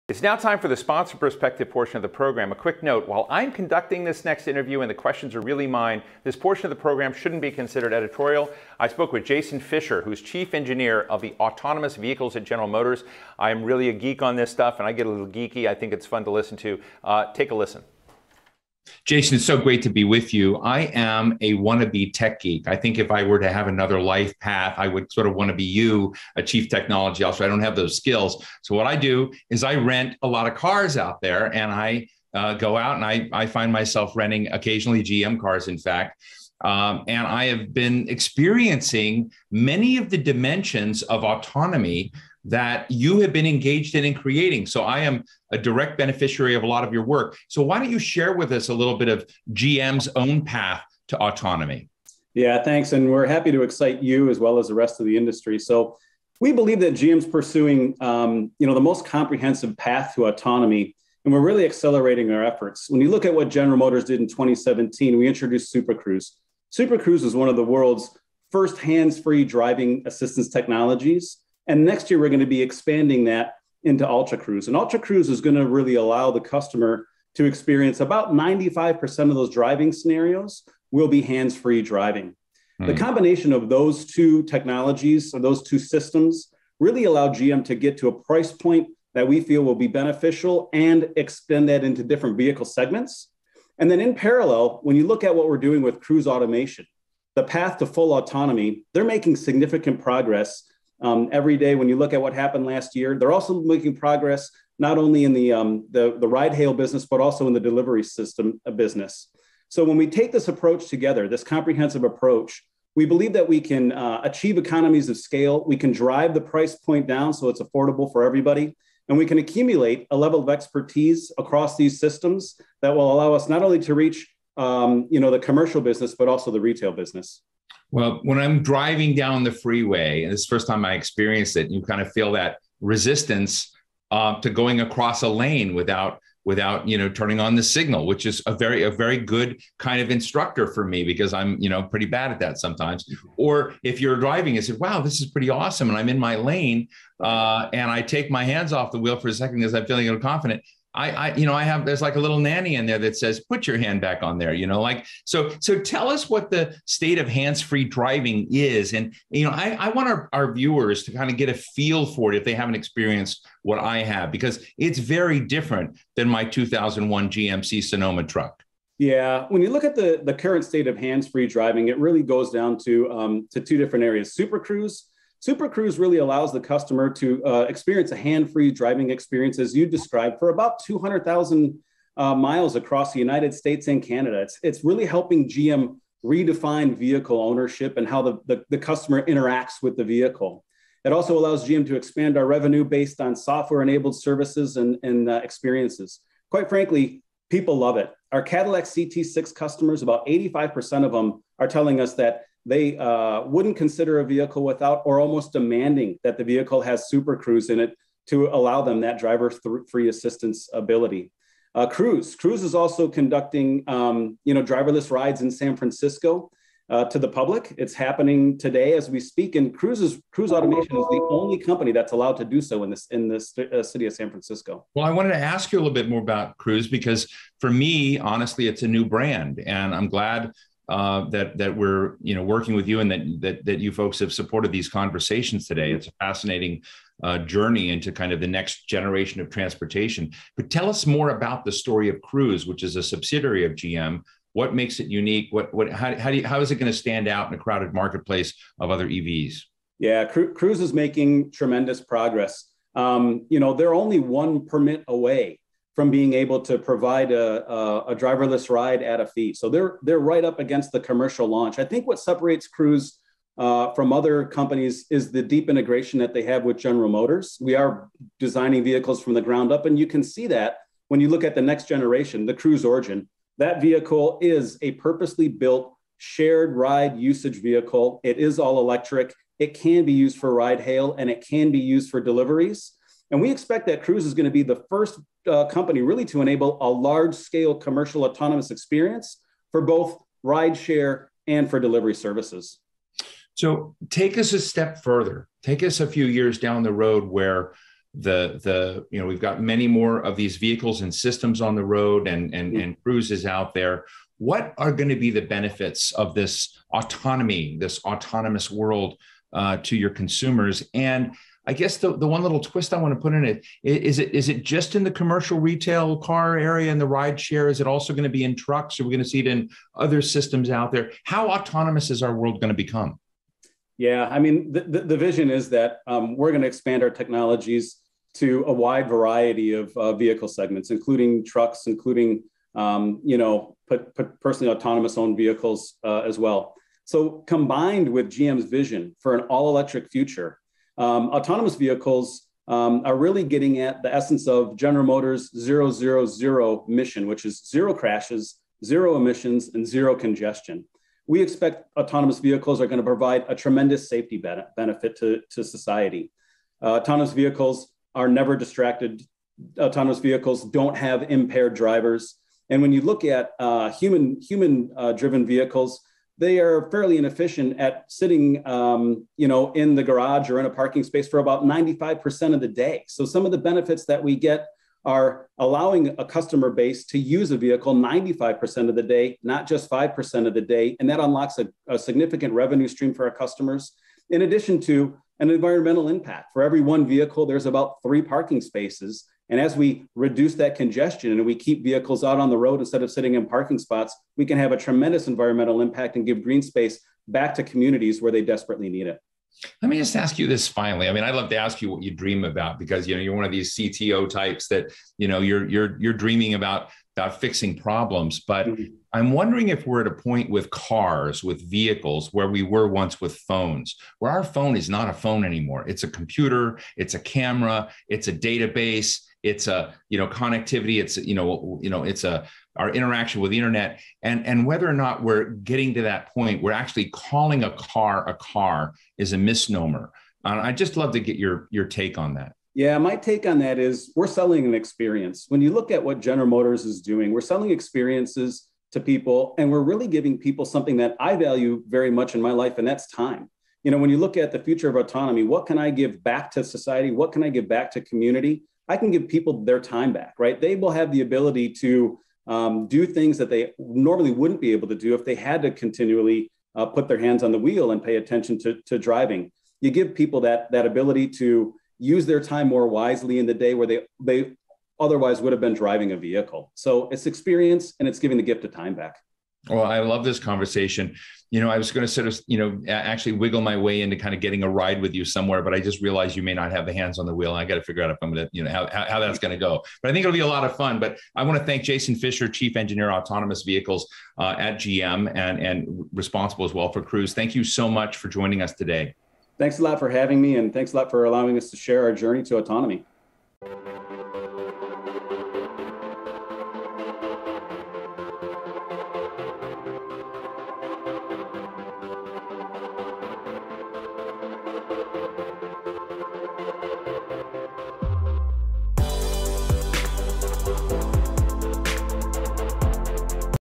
Sponsor Perspective: General Motors | Driving Tomorrow: EVs & AVs
General Motors Sponsor Perspective with Jason Fischer (Chief Engineer, Autonomous Vehicles, General Motors) in conversation with The Hill’s Steve Clemons

Driving Tomorrow: EVs & AVs, streamed on March 29, 2022.